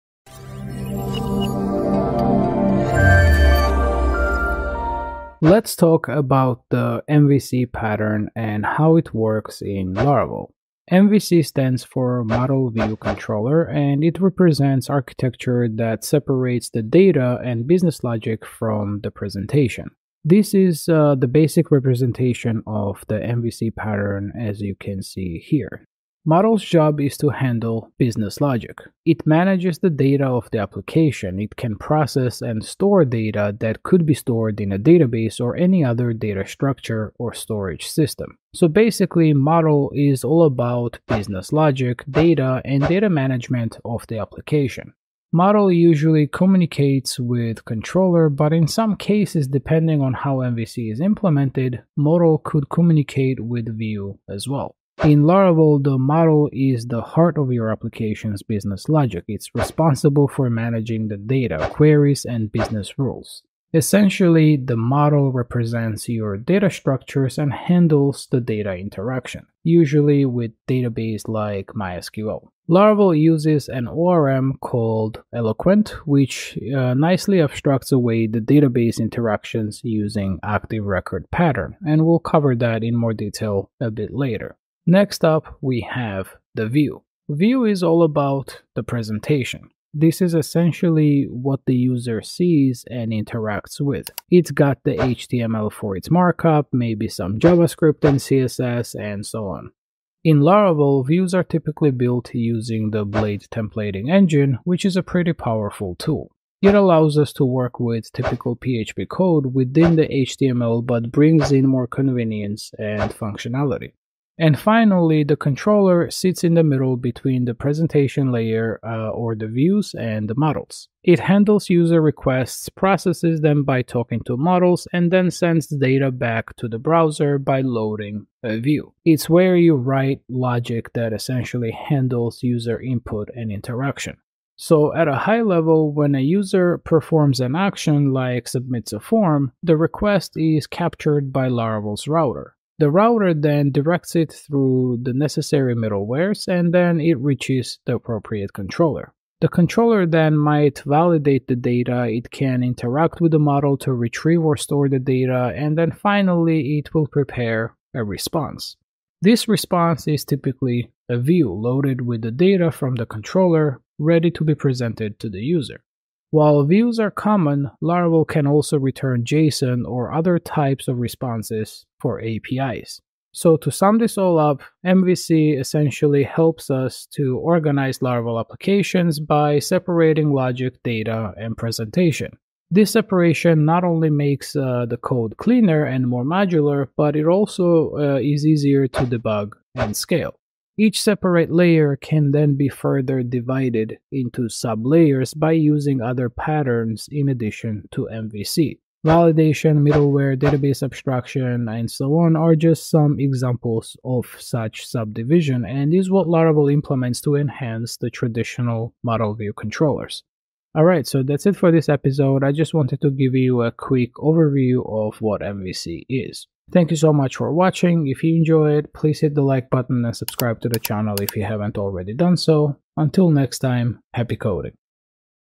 MVC the Laravel Way | Learn Laravel The Right Way
In this short episode of "Learn Laravel The Right Way," we dive into the MVC (Model-View-Controller) pattern and how it works in Laravel. We'll break down each component—Model, View, and Controller—and explain their roles in organizing your Laravel application. You'll learn how MVC separates data, logic, and presentation, making your code cleaner, easier to manage, and scalable.

     💬 Post comments, any feedback is greatly appreciated